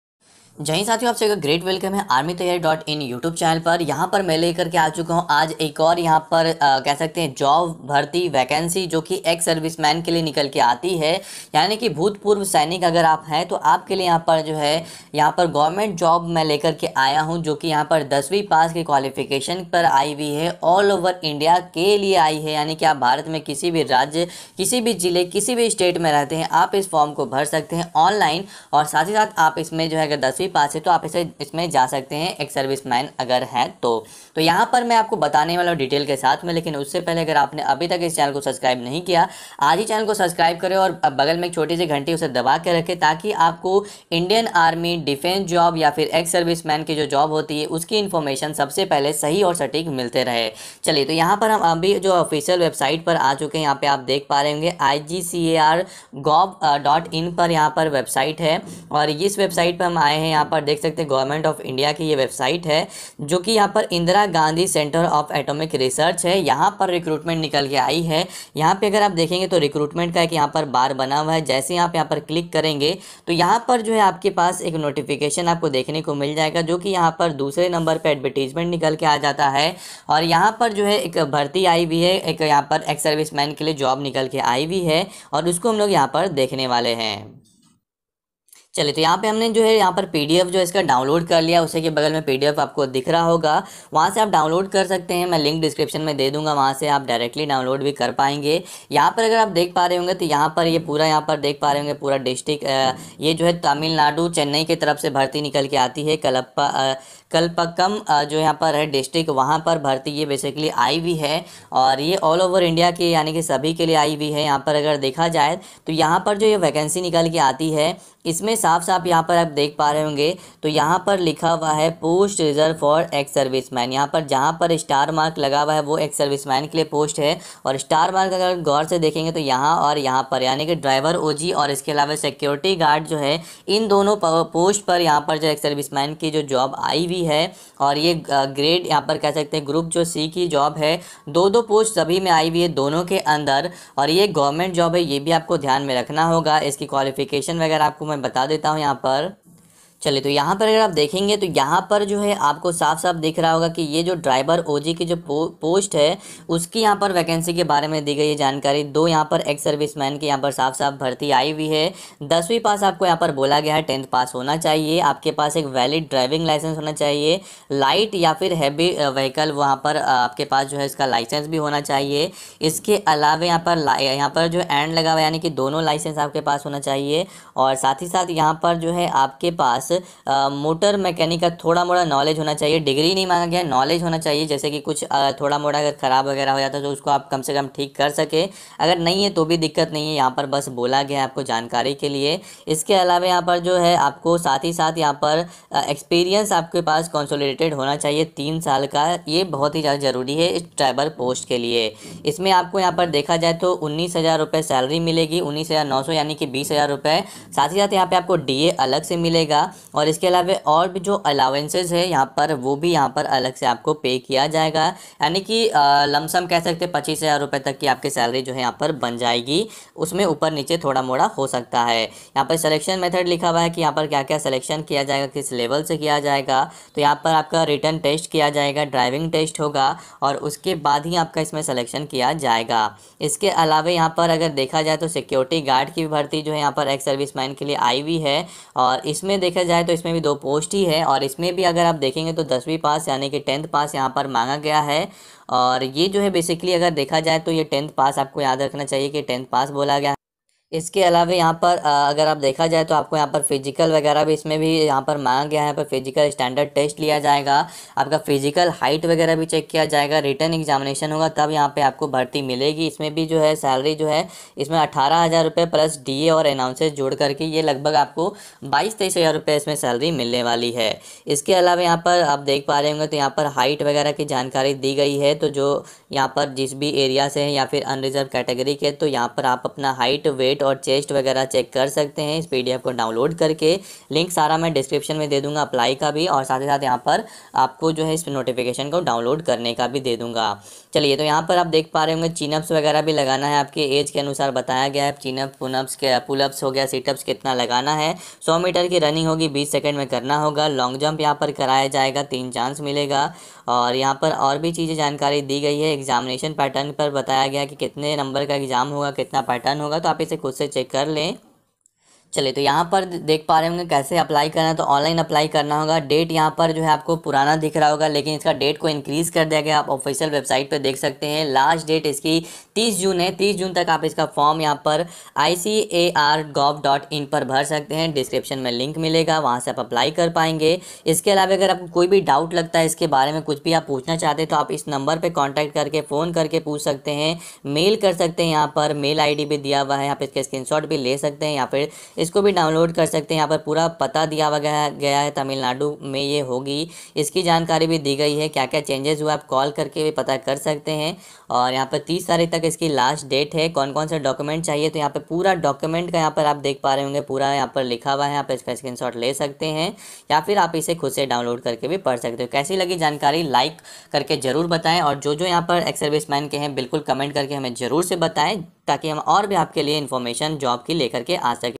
Ex-servicemen Latest Job 2022 || Ex-servicemen Job 2022 (10th pass job)
आर्मी की खुली भर्ती बिहार, उत्तर प्रदेश, हरियाणा, राजस्थान, बंगाल, सिक्किम, तमिल नाडु और पंजाब के लिए यहां देखें

job for ex servicemen, ex servicemen latest job, ex serviceman vacancy